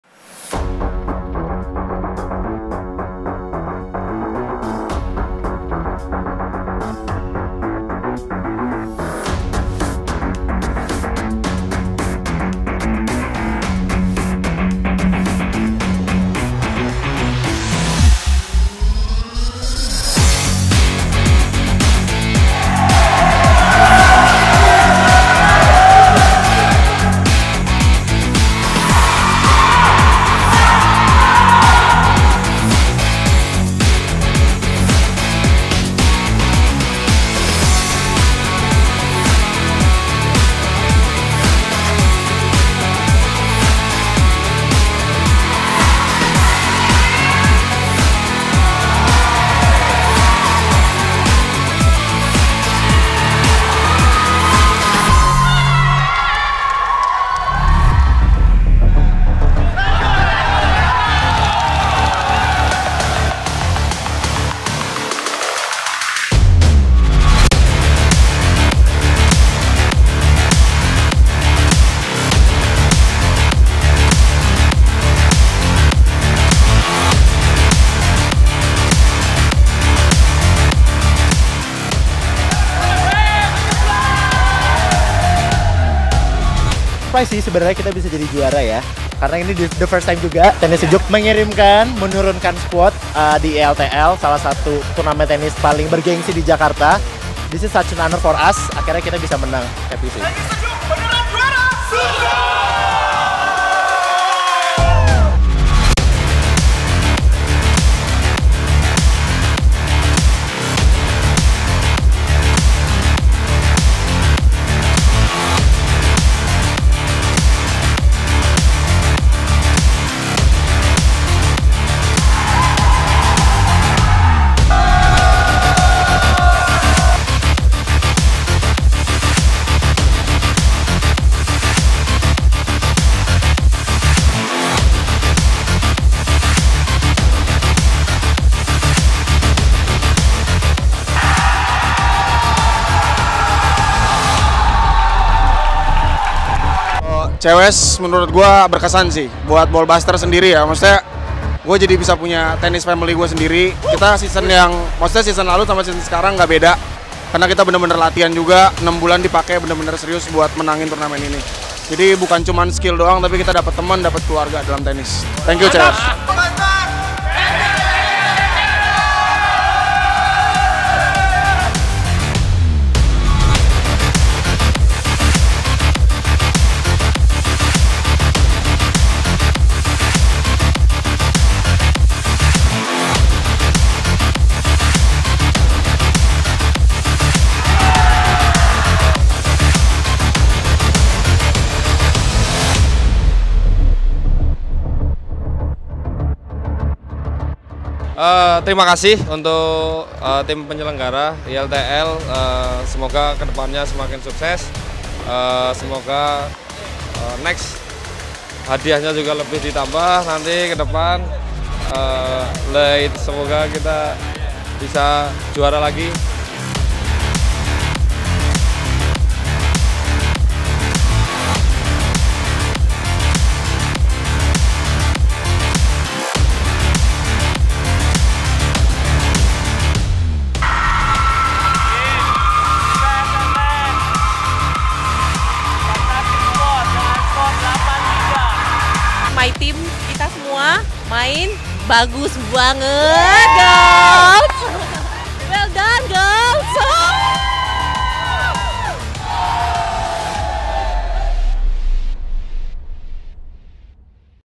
Clash of Squads Season 5 Highlights
Reliving the greatness that defined the atmosphere during Clash of Squad's 5th season! A season starting with 186 teams ended with only 6 champions. What a road to endure!

Congratulations to the new champions etched at the Grand Trophy!

Masters - ITSP
Advanced - Lockdown Prime
Intermediate - Ball Buster
Women's Intermediate - Temanis
Beginner - Tennis Sedjoek
Women's Beginner - Funtastic Squad

We look forward to seeing you compete in the next season!

Compete at the highest level and find new friends in the biggest tennis league in Indonesia.